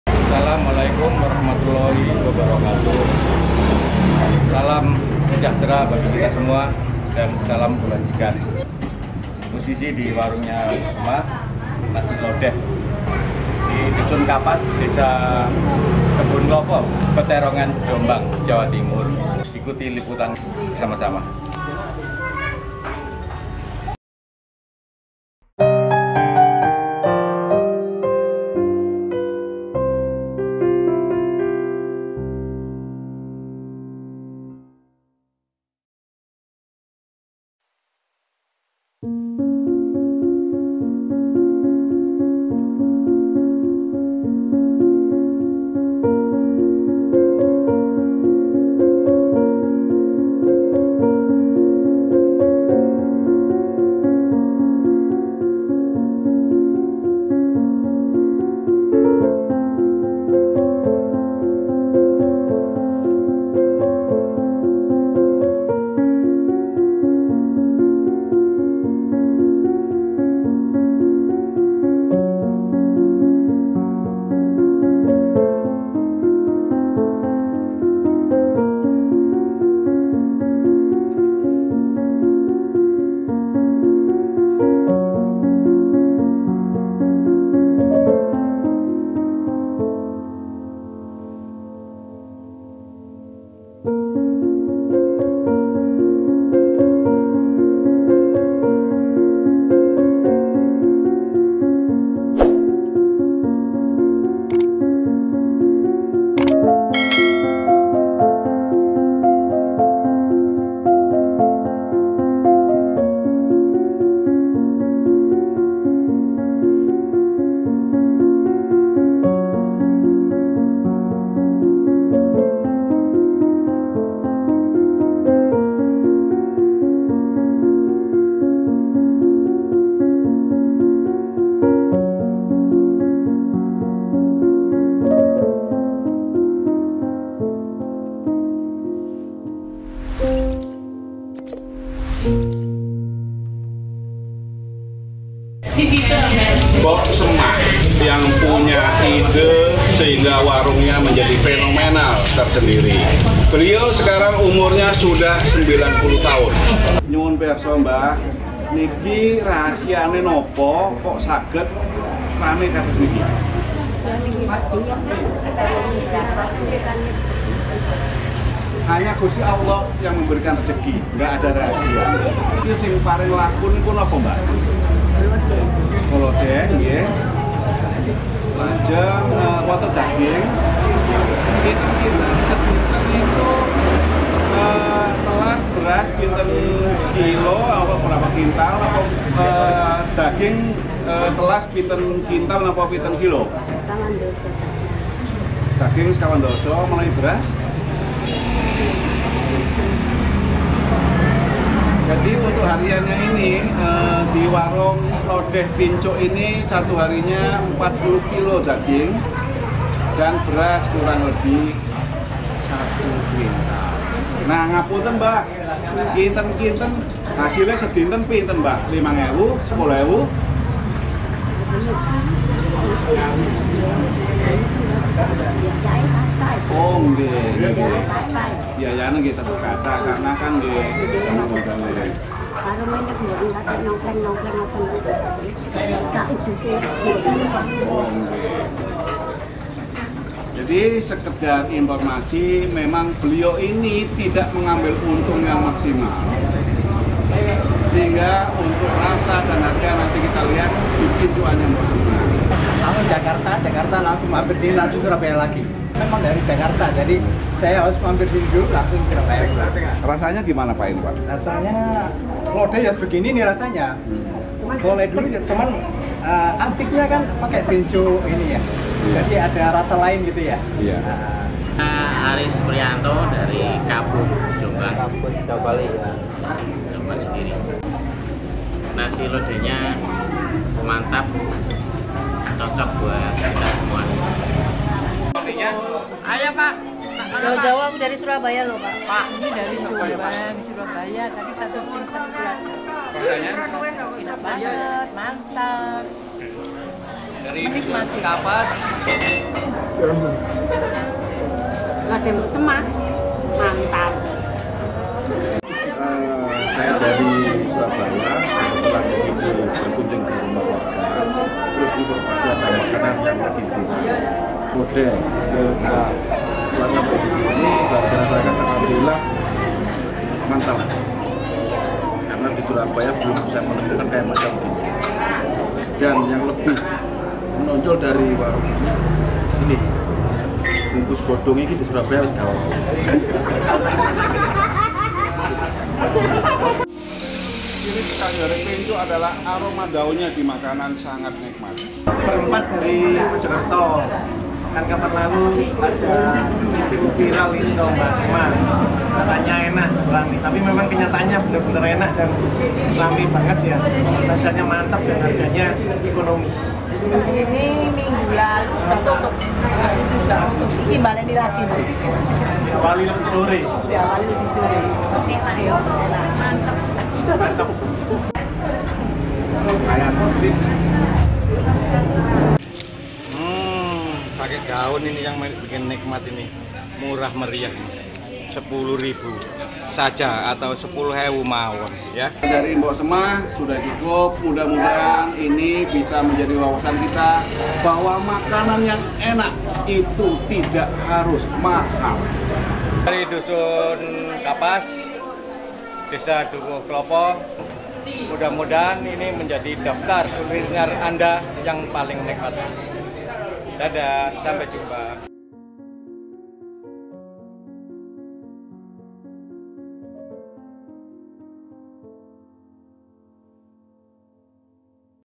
SEGO LODEH PINCUK MBOK SEMAH : OMSET LEBIH 1,5 M TAHUN
MBOK_SEMAH, merintis usaha sejak tahun 1980-an. Dari awal menu andalannya adalah "sego lodeh", makanan rakyat kelas bawah dengan harga terjangkau. "PINCUK" dijadikan alas makan, karena piring harganya terlalu mahal ukuran waktu itu.

Seiring waktu, pincuk itulah yang menjadi "trade" sego lodeh Mbok Semah.

Seiring waktu pula, lauk pauk awalnya  tahu-tempe, berubah menjadi lauk mahal : daging, paru, jeroan sapi. Disamping pincuk yang tidak berubah adalah : krupuk ! Kombinasi inilah menjelma dari makanan rakyat, menjadi makanan seluruh lapisan masyarakat dengan segudang kenikmatan dan memori didalamnya.

Keunggulan ini tidak membuat kepongahan dalam hidupnya. Beliau tetap sederhana. Harganyapun dipatok murah. Lodeh+krupuk harga Rp. 6.000. Lodeh+krupuk+lauk daging Rp. 10.000. Jeruk anget, dengan tetap penuh rasa jeruk, harga Rp. 2.000.

Latar belakang hidup sederhana, membuat ukuran ekonomi mbok Semah lebih mementingkan perasaan "saling", dibanding keuntungan materi.

Host : Suyitno
Juru Kamera : Afid Kurniawan, Sugeng, Sofyan Lodji
Editor : Budiono A

Info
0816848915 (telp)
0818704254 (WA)